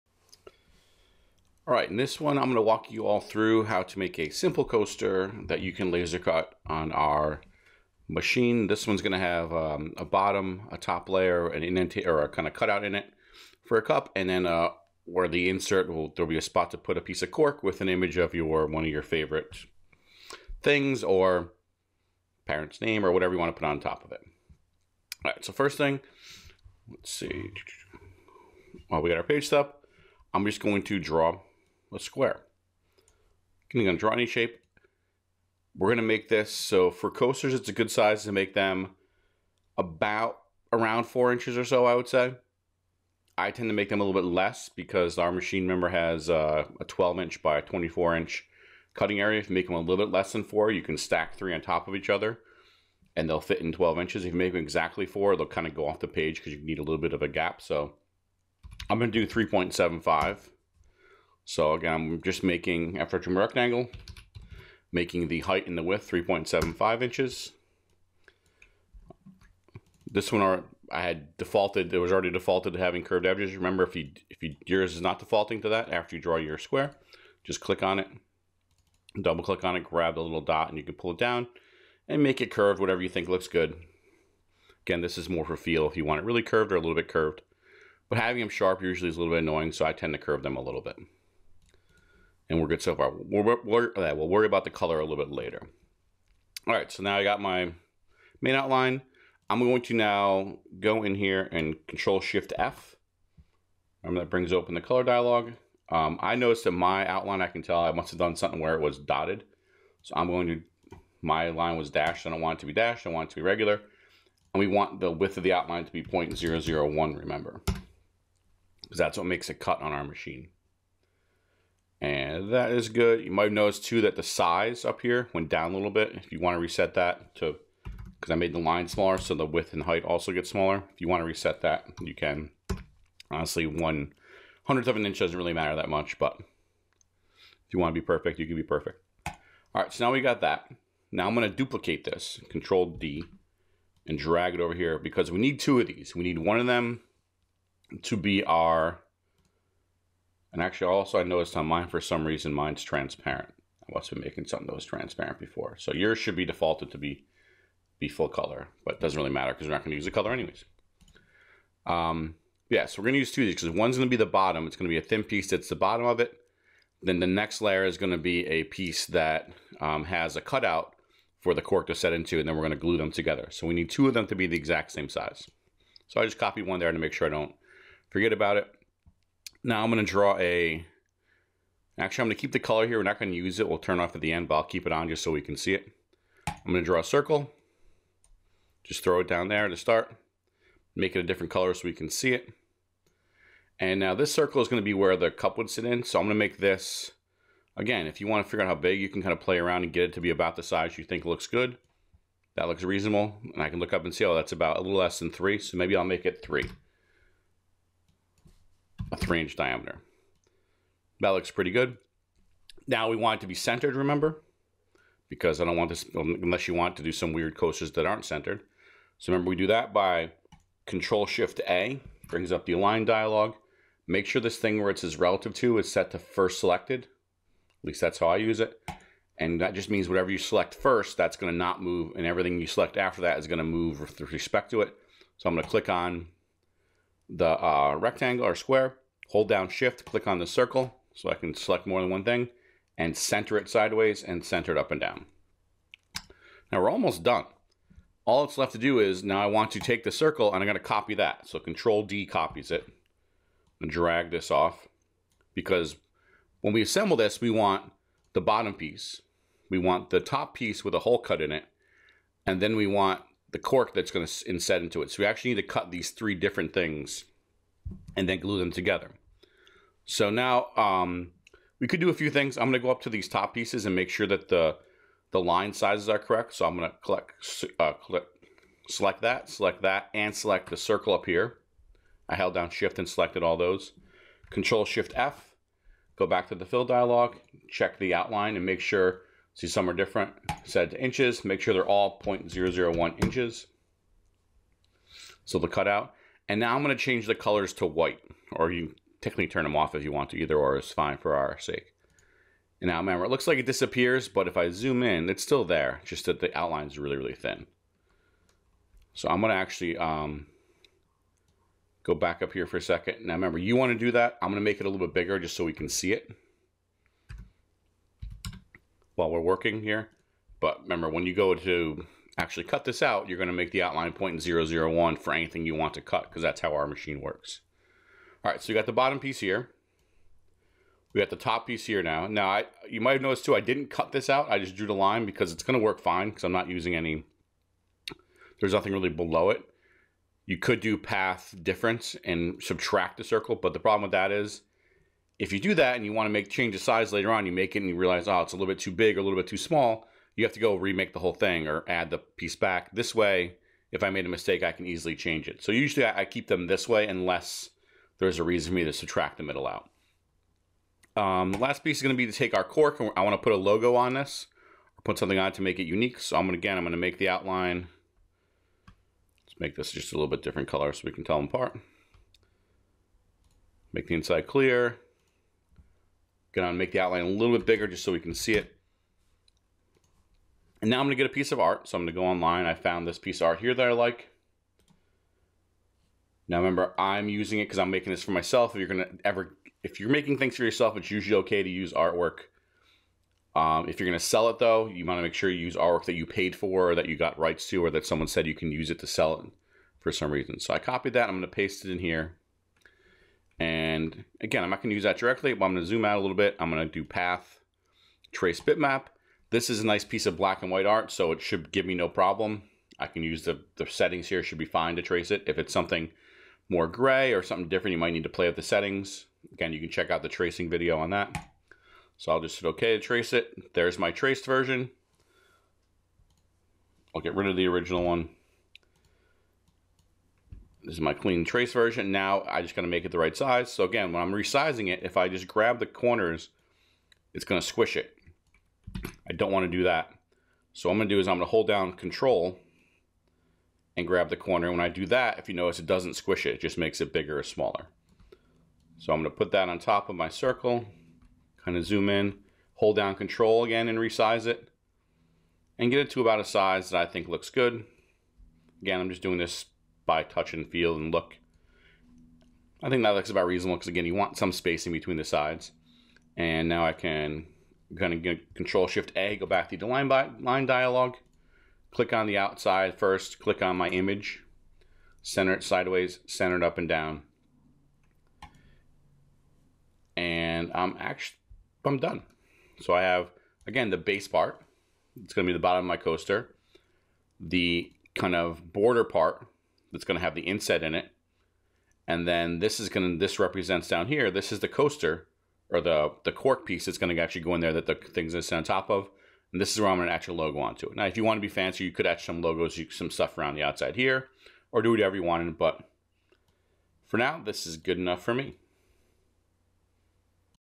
Inkscape Tutorial: Inset Coaster
This will show you how to design a coaster with a cork inset and a logo on it to be lasercut.